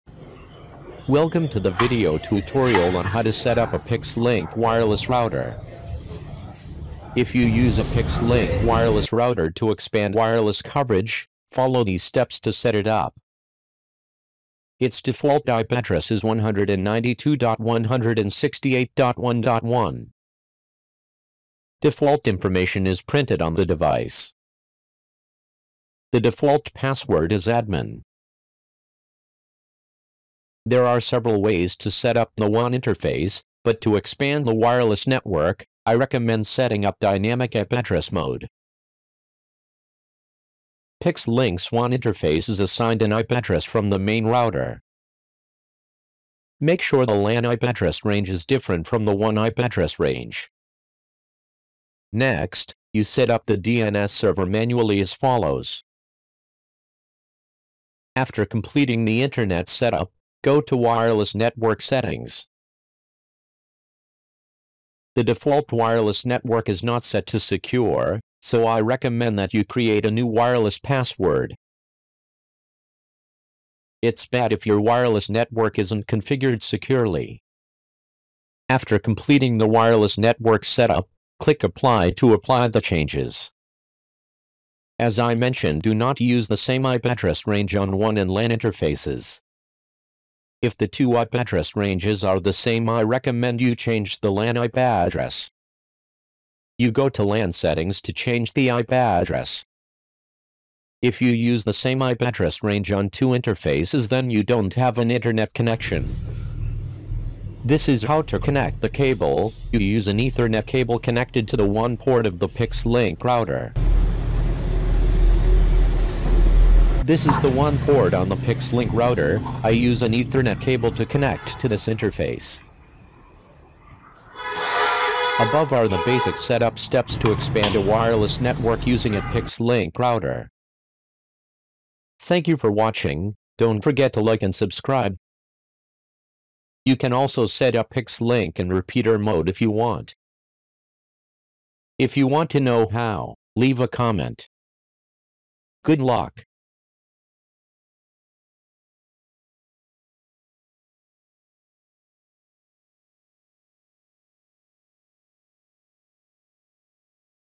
Pix Link 192.168.1.1 | How to Setup PIX-LINK Wireless Router
In this video, I will show you how to easily set up your Pix-Link wireless router. This video will walk you through the process of connecting your router to the internet, configuring the settings, and securing your network.